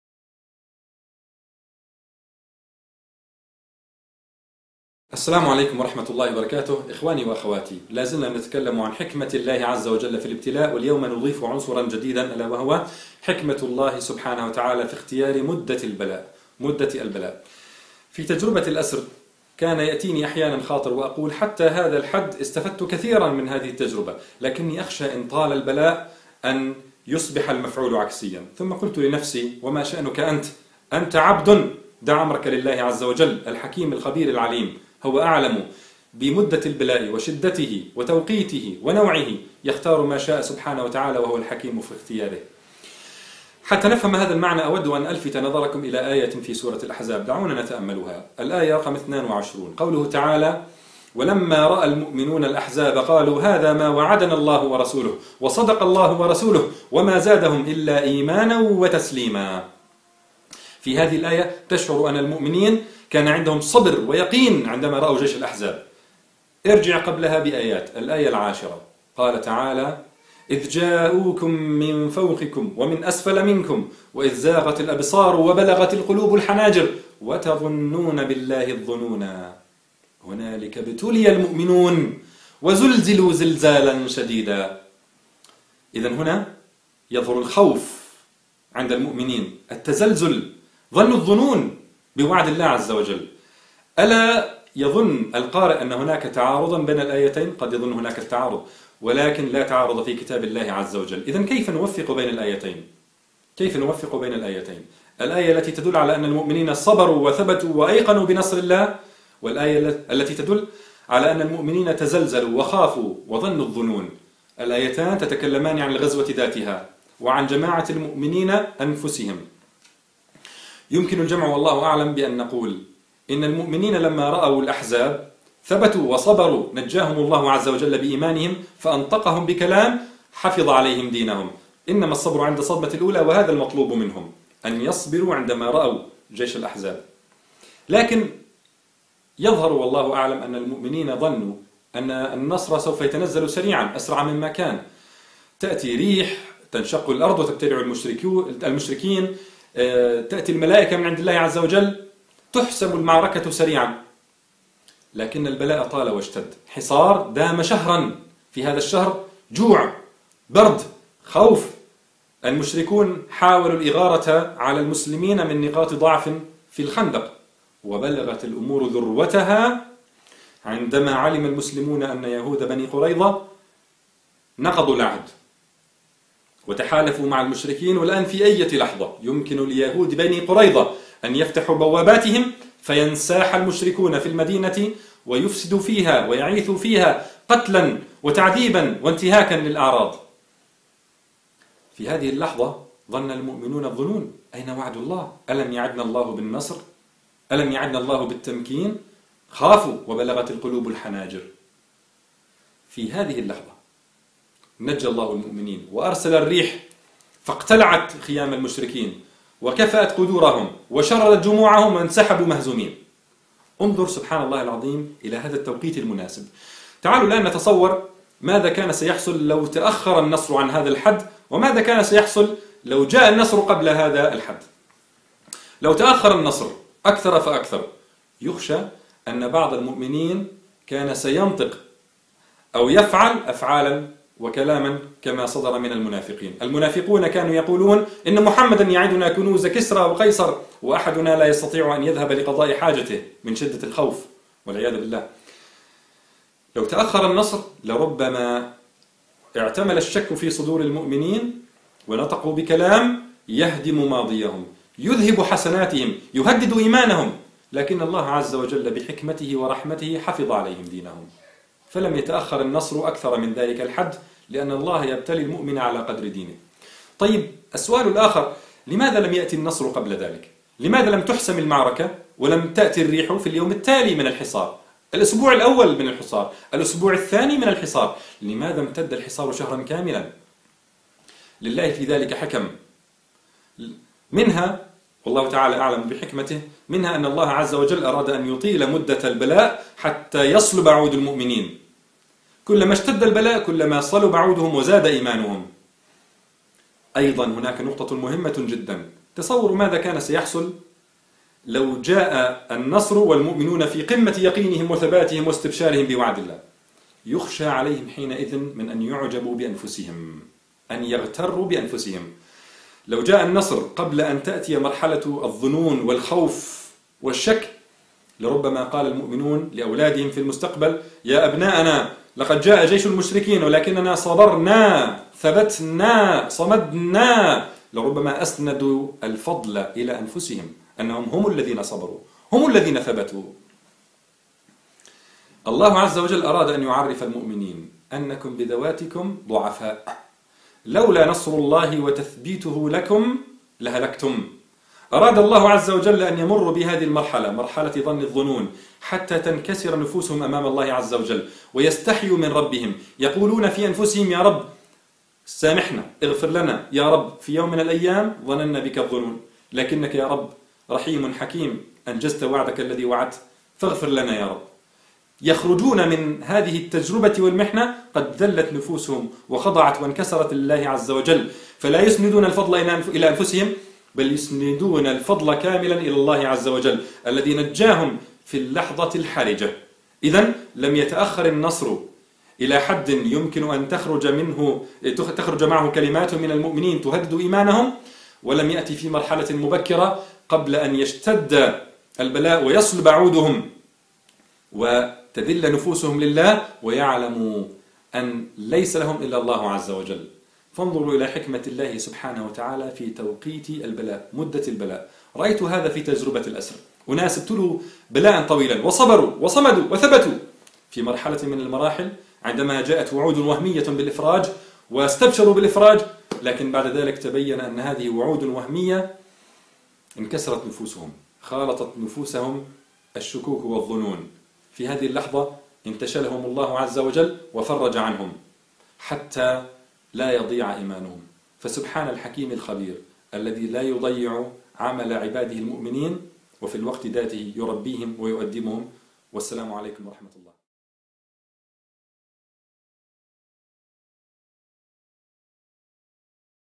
فن حسن الظن بالله ٨: ستفرج في اللحظة المناسبة! - د. إياد قنيبي
تعالوا نتأمل حكمة الله تعالى في تقدير مدة البلاء، كيف يربي عباده ويثيبهم ويعلمهم الصبر، لكنه لا يطيل البلاء إلى حد يضيع معه المؤمن ما حصله من أجر في محنته.
كيف يزيل الله تعالى الغرور من نفوس المؤمنين فيمنعهم من أن يعجبوا بأنفسهم وينسبوا الفضل في الصبر إلى ذواتهم؟ الإجابة في هذه الحلقة من سلسلة فن إحسان الظن بالله تعالى.